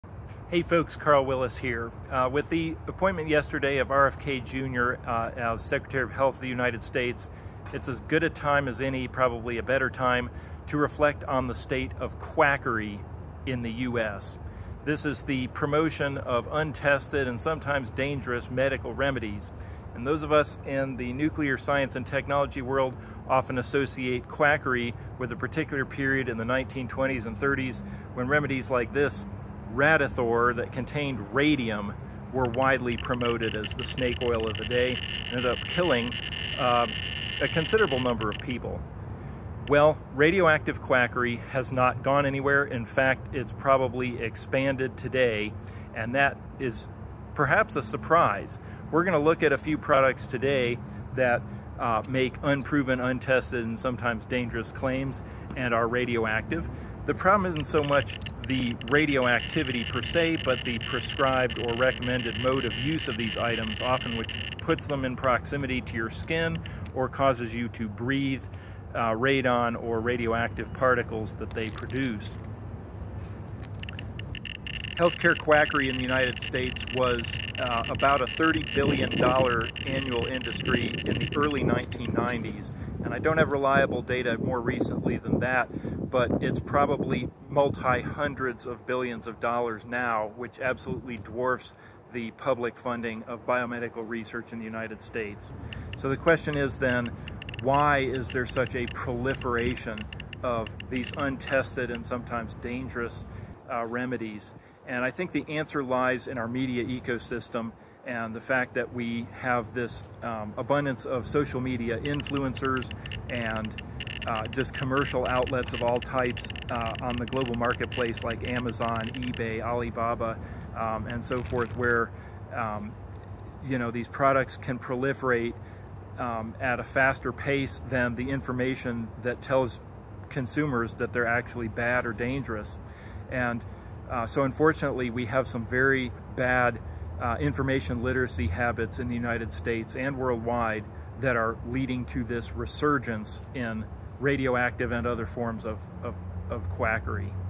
Contemporary radioactive quackery
A day after a notable quack was confirmed as the new US Secretary of Health and Human Services, I take this opportunity to quickly look at a few examples of radioactive quack remedies widely available right now on American e-commerce sites where they are sold as alternative health and wellness accessories.  The modern landscape of radioactive quackery differs from the "golden age" in the 1920s and '30s in that the radioactive items sold now are never promoted as such: they hide their risks (and illegality) behind language suggesting they are sources of negative ions, unspecified energy, qi, etc.  Typical modes of use involve wearing the items, sleeping with them, and breathing through them, all of which constitute risky exposure to the material they contain (most often, thorium).  The resurgence in radioactive quackery parallels the rise of an ecosystem of social media influencers promoting unscientific ideas and untested products, eroding the attention and respect formerly given to scientific understanding.